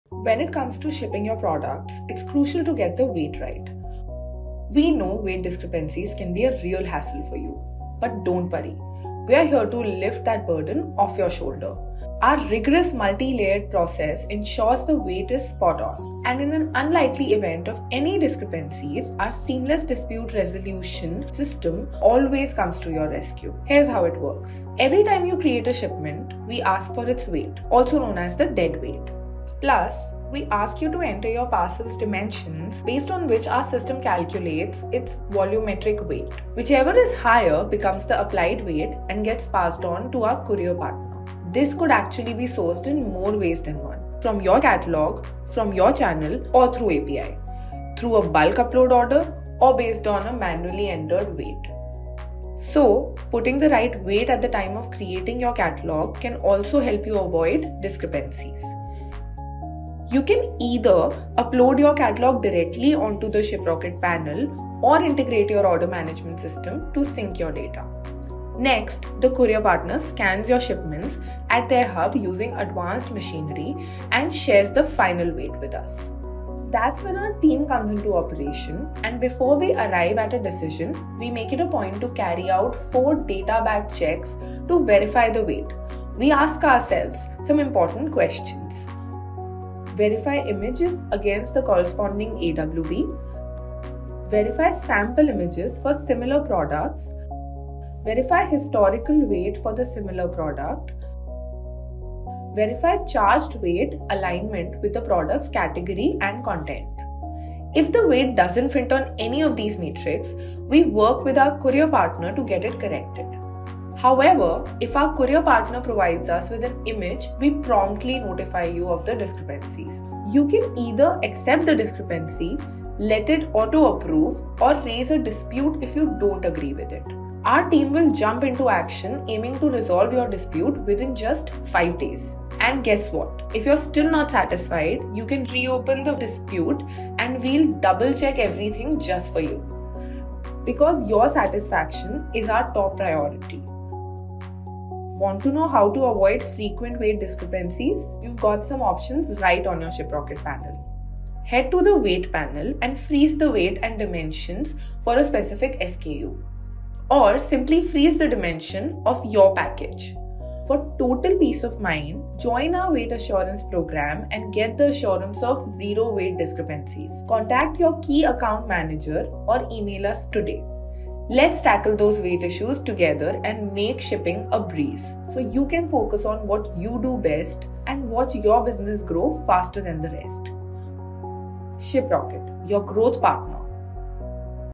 Get The Ultimate Solution To Weight Discrepancies With Shiprocket!
Ever felt burdened by weight discrepancies? To end this struggle once and for all, we've innovated and developed a groundbreaking solution! ⚖️📦

Explore how we’re ensuring precise weights, conducting data-driven verification checks, and resolving disputes real quick. Plus, enjoy the assurance of zero weight discrepancies.

Dive into the video for the details!